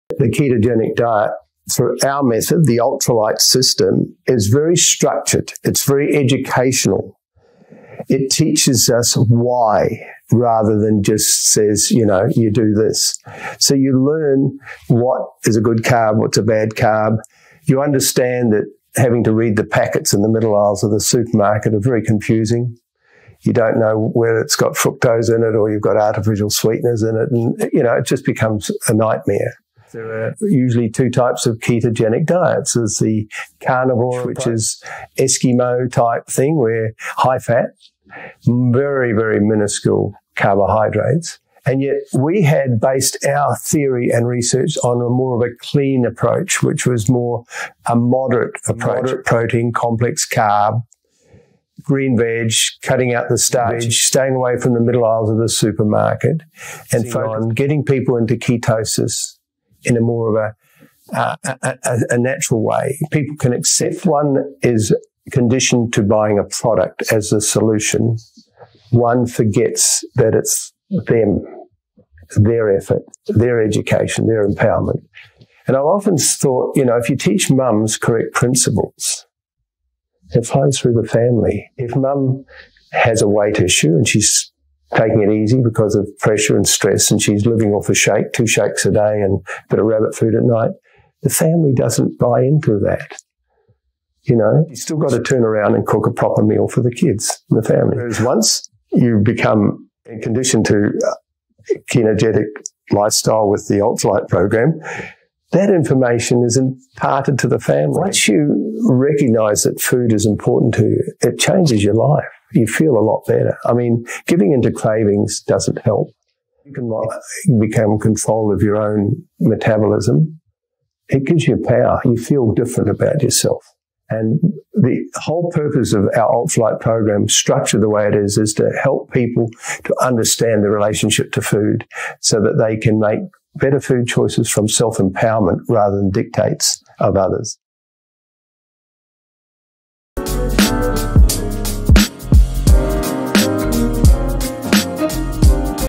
Why is the Ultra Lite Ketogenic Diet the best way to lose weight and feel amazing
The education you receive from following the Ultra Lite Keto Program will guide you in how to make long lasting healthy decisions, which will not only impact your health but your family members as well.

to find out more about our life changing program today!

When: Jun 7, 2022 07:00 PM Canberra, Melbourne, Sydney
Topic: Help Your Clients Lose Weight and Stay Healthy with the Ultra Lite Keto Program